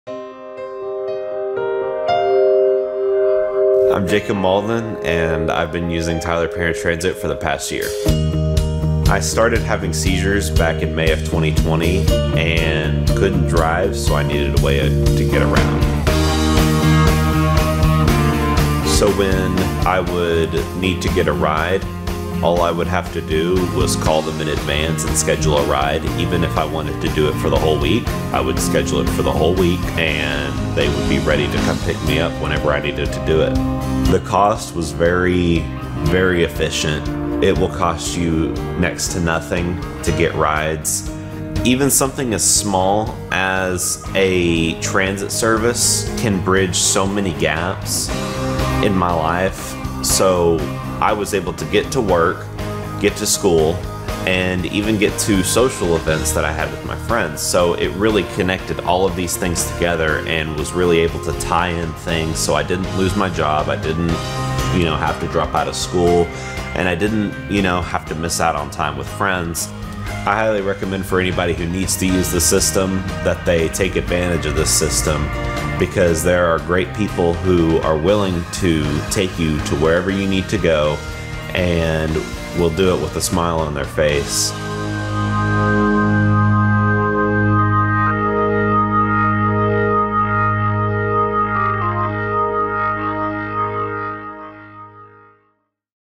Tyler Paratransit - Jacob Mauldin
Jacob Mauldin tells how he kept up with school, work and relationships after a life changing diagnosis thanks to Tyler Transit's Para Transit service.